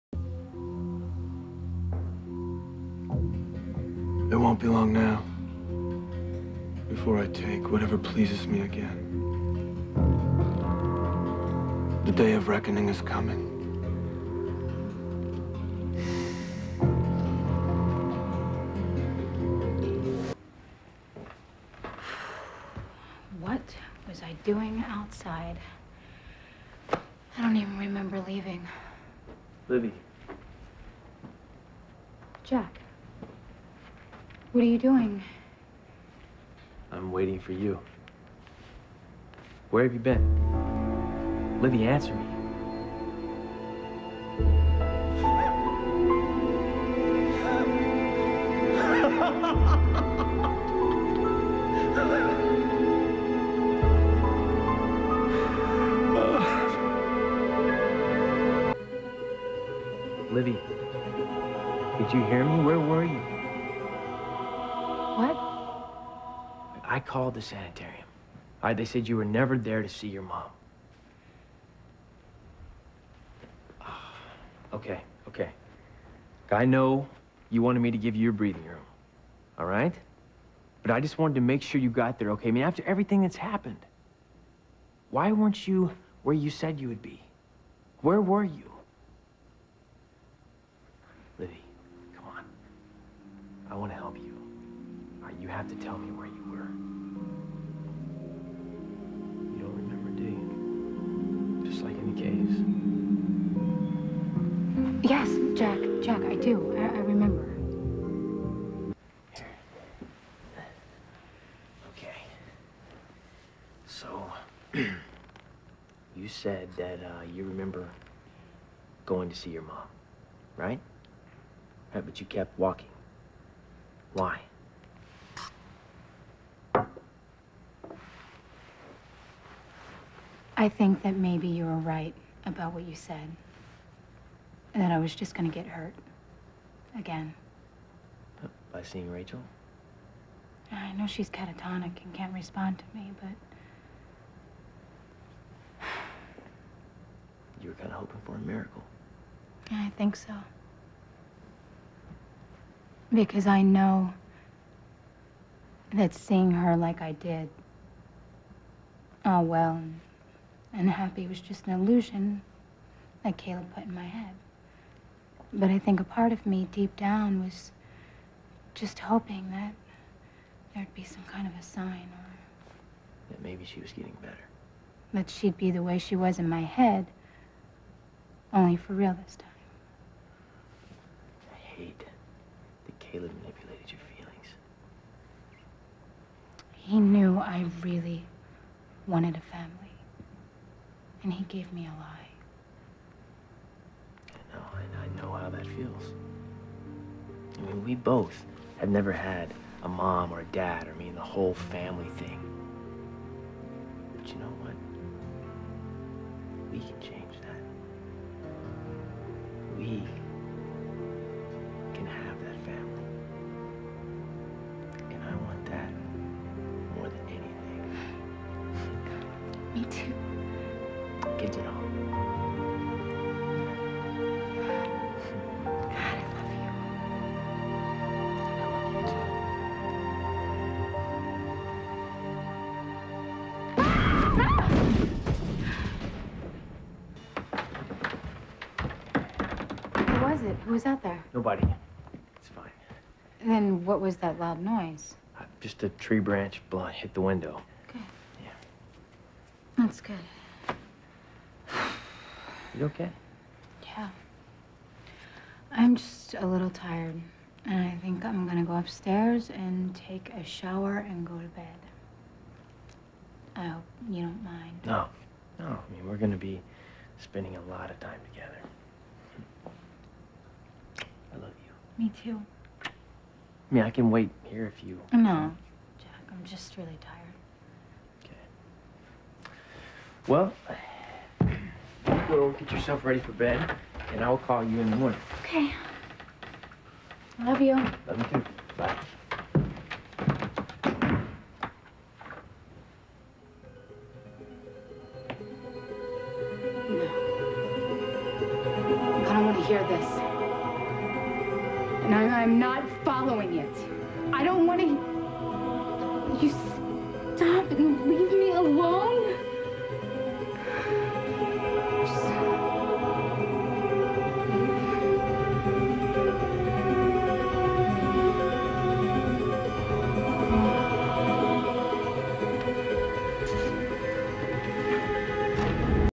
Forgotten- TE32- Tempted
Livvie can't remember, and some strange occurences take place while she's with Jack...while Caleb listens in...